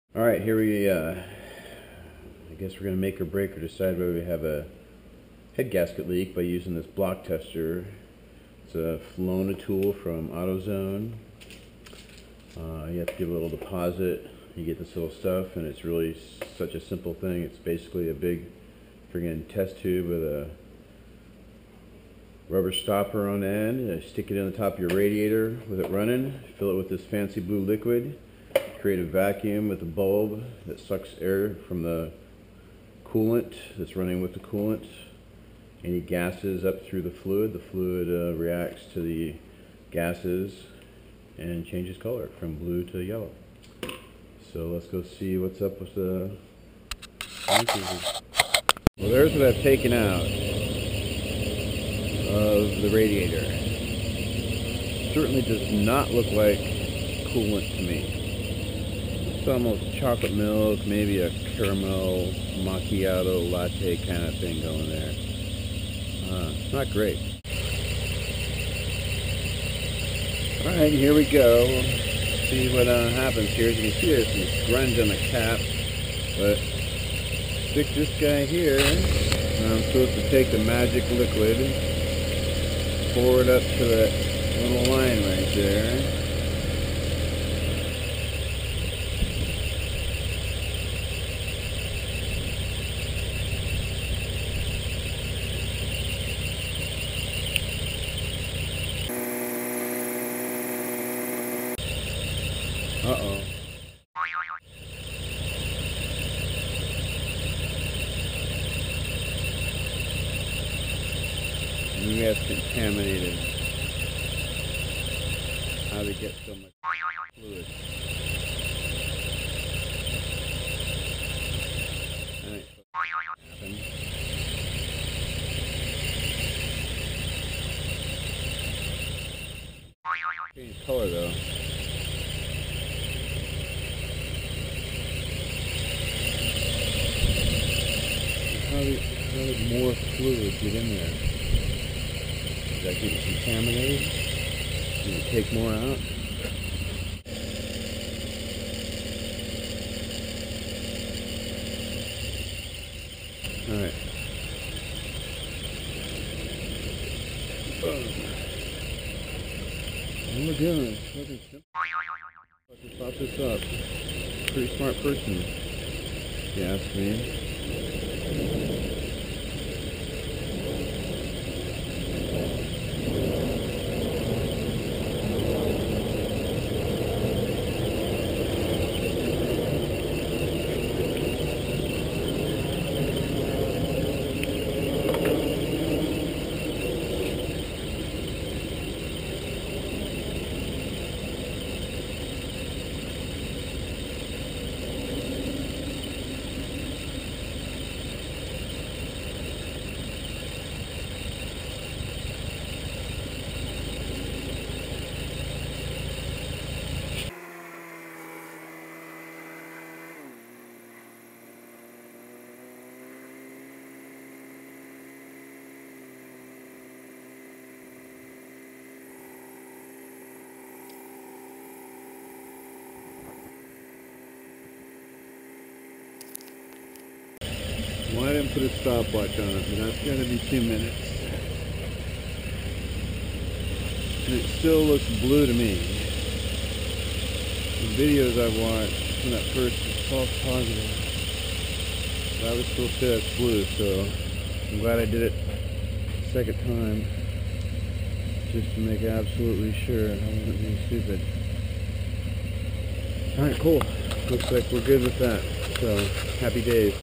engine block test
Test performed for a leaking head gasket or cracked block.  The test uses a kit from Autozone in the tool loaner program, no cost.